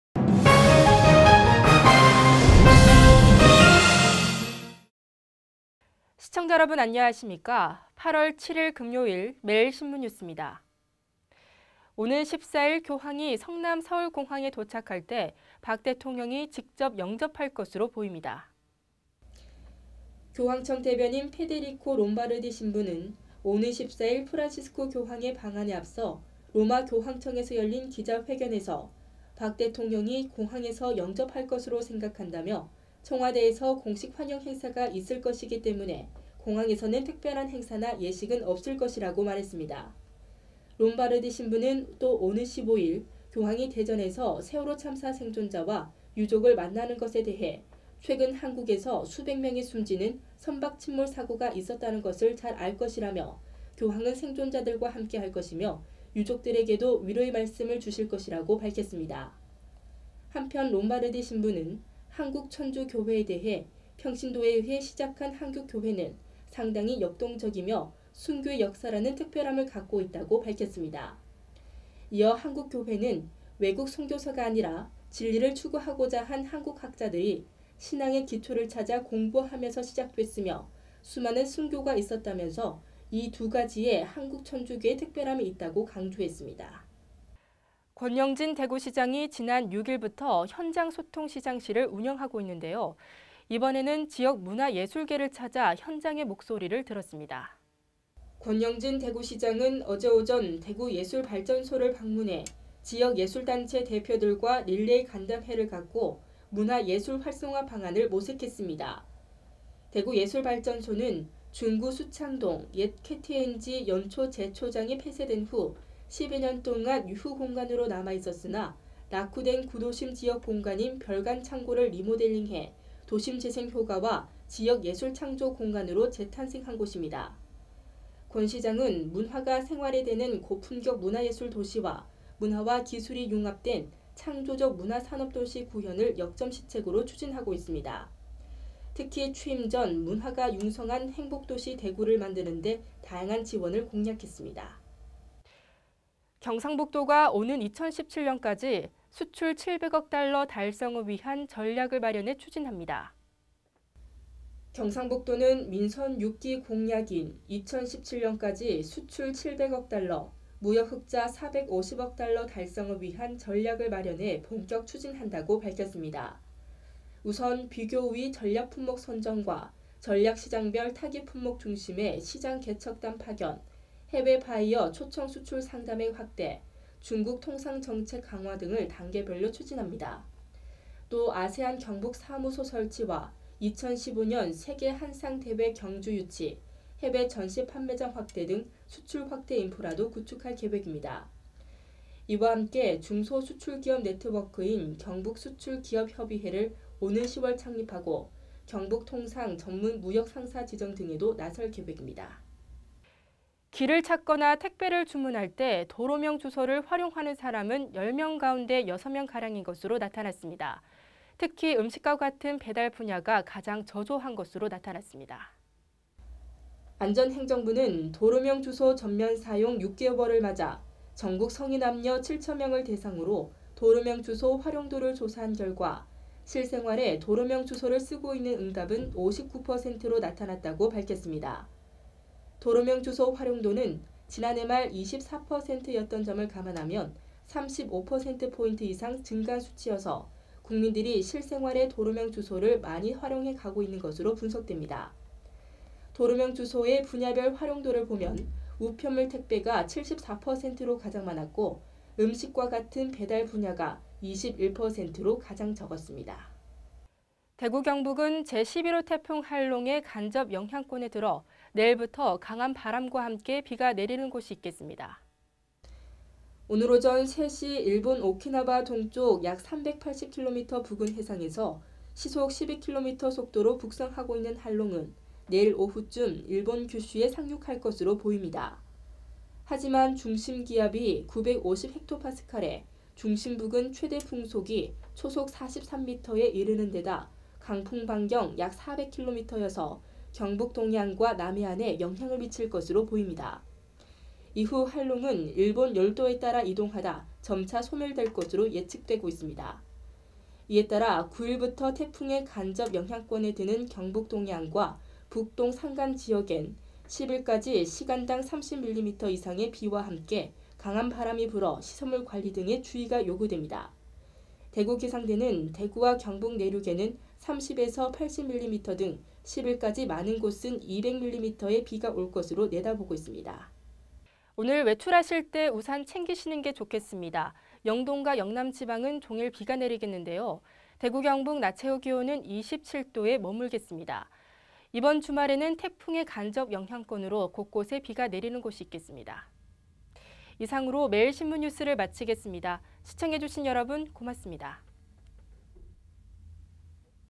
매일영상뉴스 0808
-교황 방한때 박 대통령이 공항서 직접 영접
-권영진 대구시장, 지역 문화예술계와 소통
-경북도 '2017년까지 수출 700억달러 달성' 전략 추진
-국민 59% 도로명주소 활용…음식배달시 사용 저조
-주말 대구경북 태풍 할롱 간접 영향권